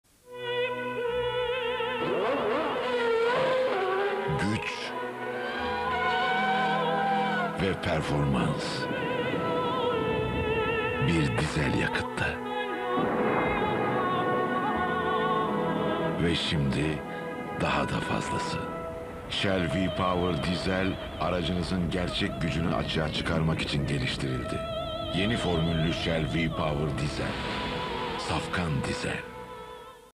Shell V-Power Diesel Reklamı 2005
Selçuk Yöntem'in sesinden Shell reklamı